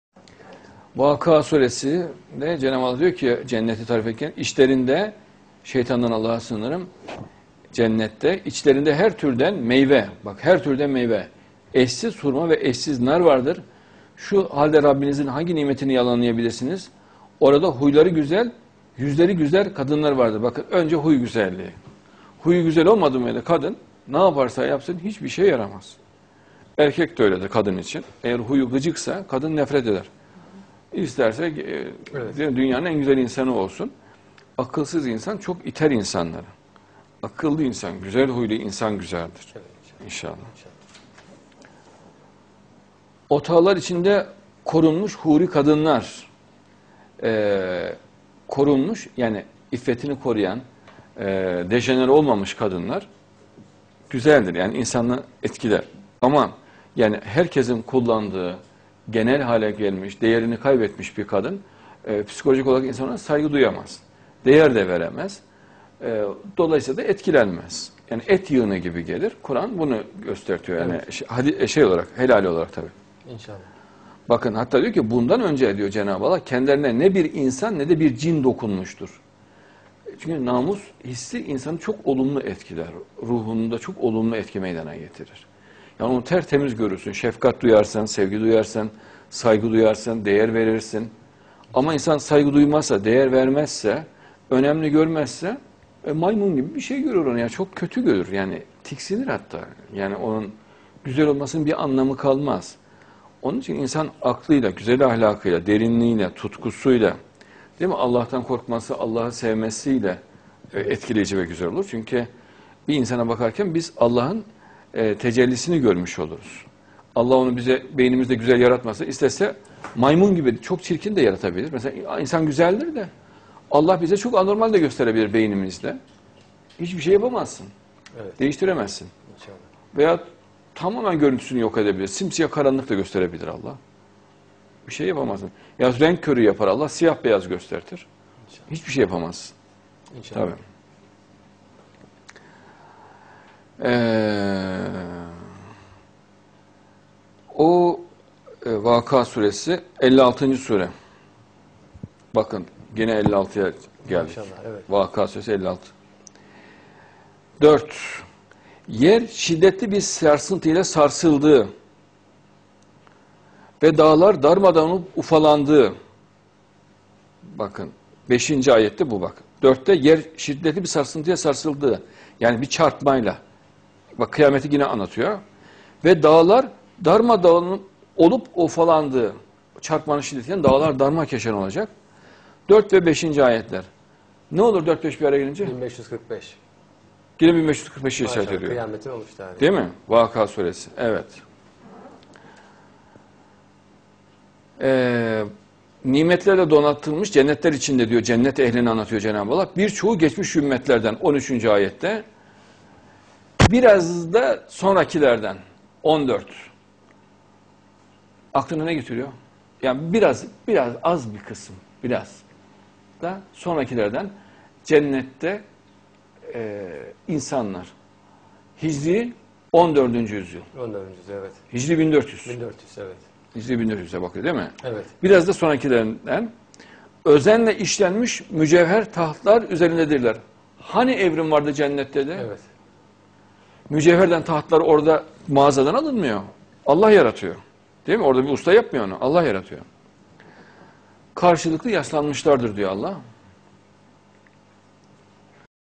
HARUN YAHYA - Explications de la sourate Ar-Rahman et de la sourate Al-Waqi'a
HARUN YAHYA

Explications de la sourate Ar-Rahman et de la sourate Al-Waqi'a: Même si on est la personne la plus belle sur terre, si cette personne n'a pas de bonnes mœurs, alors elle est répulsive. Des références à l'heure du Jour du Jugement et autres sujets

(Extrait de l'interview d'Adnan Oktar en direct sur Tempo TV et Kral Karadeniz TV, le 1er janvier 2010)